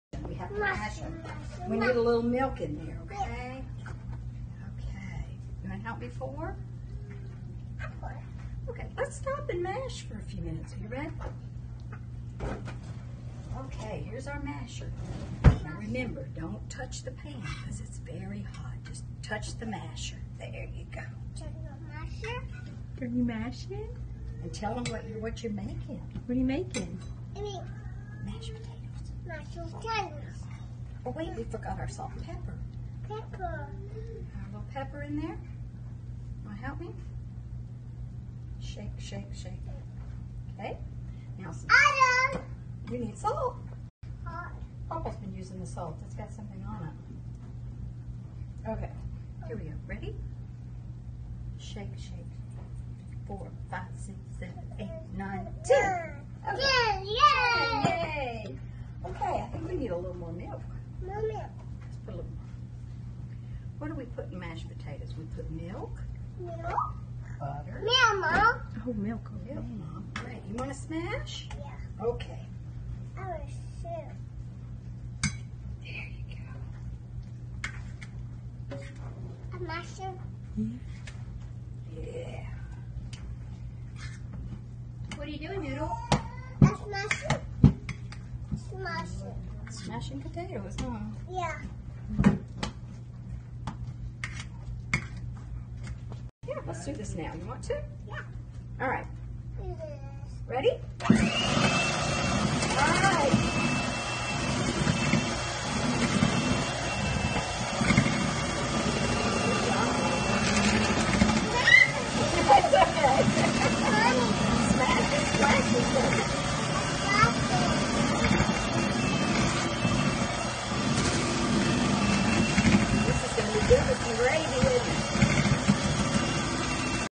Toddler Cooking Lessons With Mawmaw!!
Helping Mawmaw cook dinner. “Mashed taters”